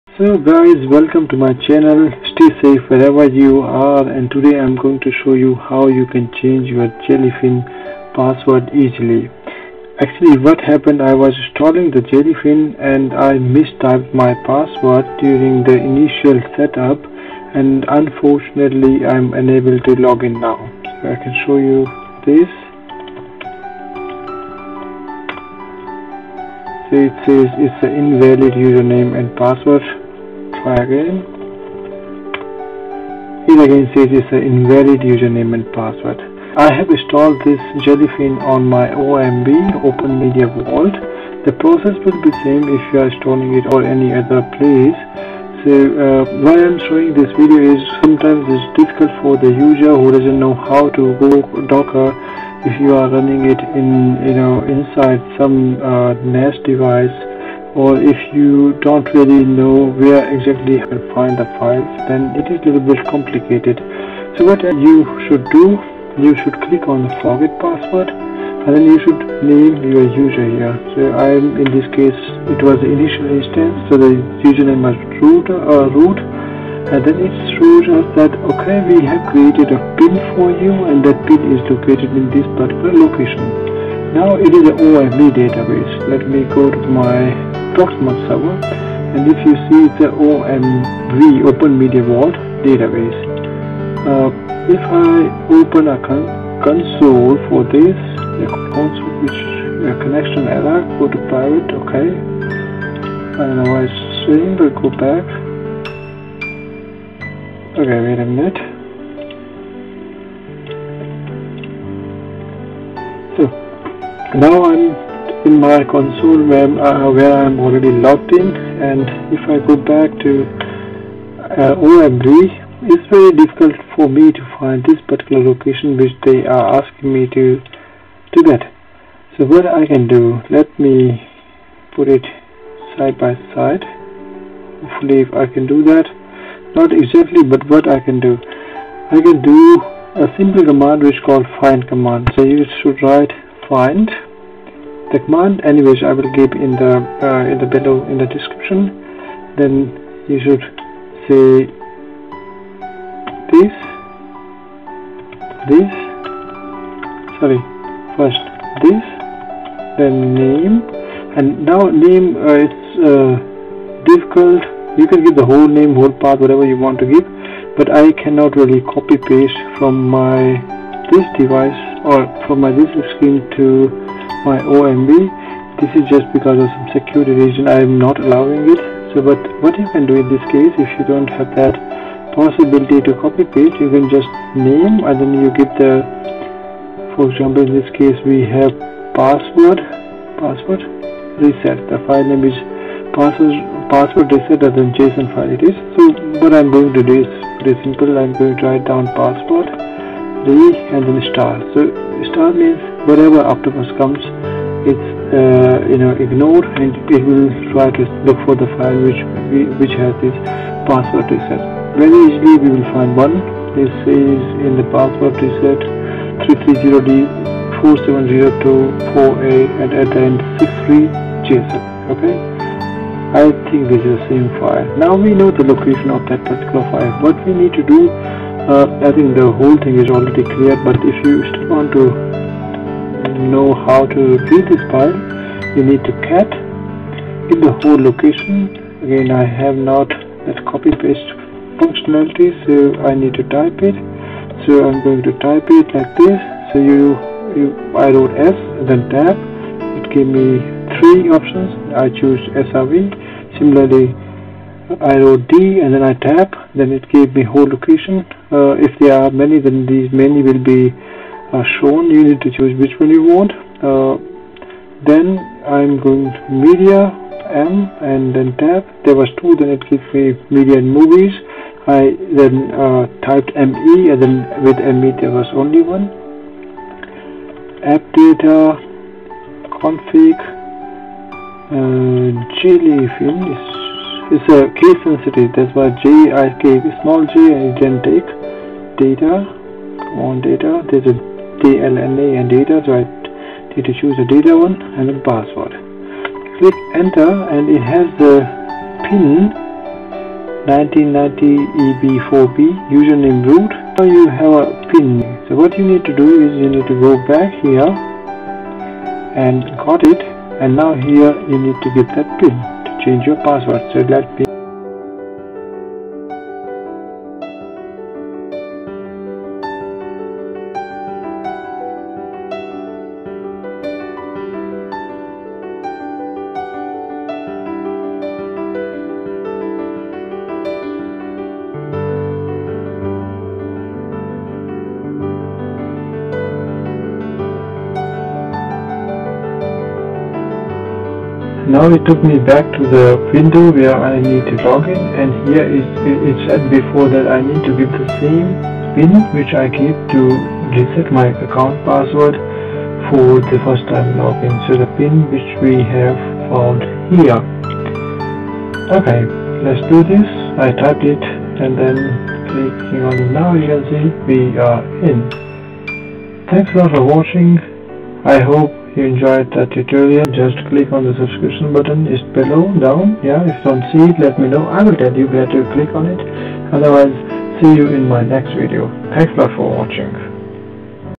Jellyfin password change in docker or NAS Device (Openmediavault)
Jellyfin password change in docker or NAS Device . I am using Openmediavualt but the procedure will be same for others . There are many ways of doing it. This method will be helpful if you are not aware about how docker works.

Command
find / -name passwordre*